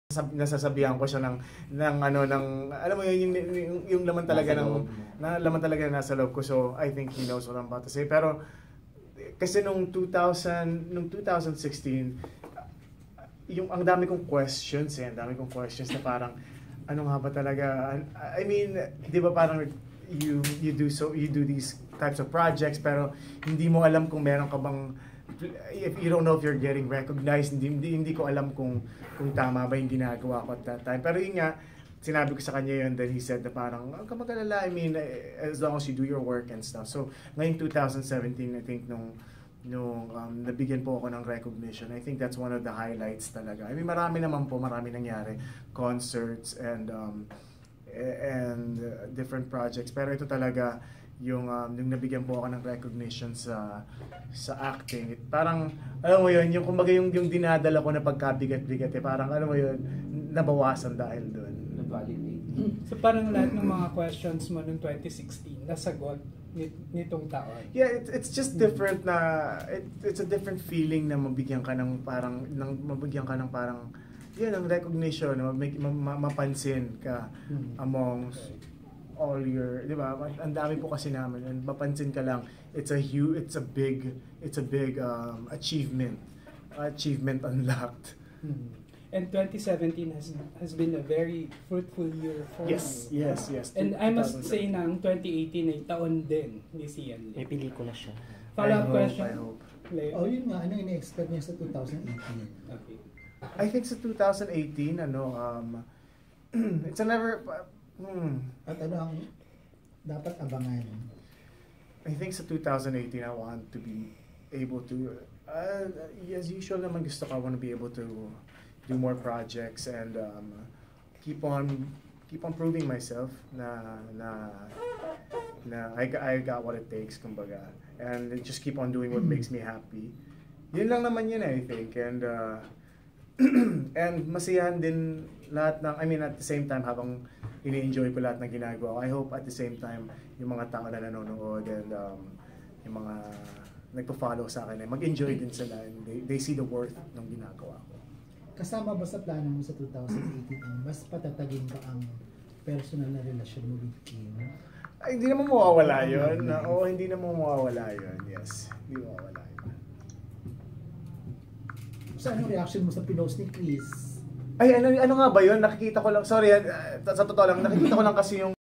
Xian's realizations on 2017 and answered questions
Key of X by Xian Lim Dec6 at Eastwood City Park